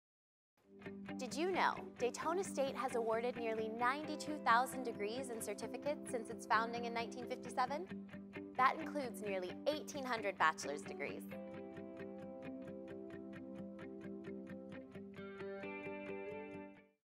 Did you know, DSC has awarded nearly 92,000 degrees and certificates since its founding in 1957?
Did you know, Daytona State College has awarded nearly 92,000 degrees and certificates since its founding in 1957. That includes nearly 1,800 bachelor’s degrees since the college began offering them during the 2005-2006 academic year.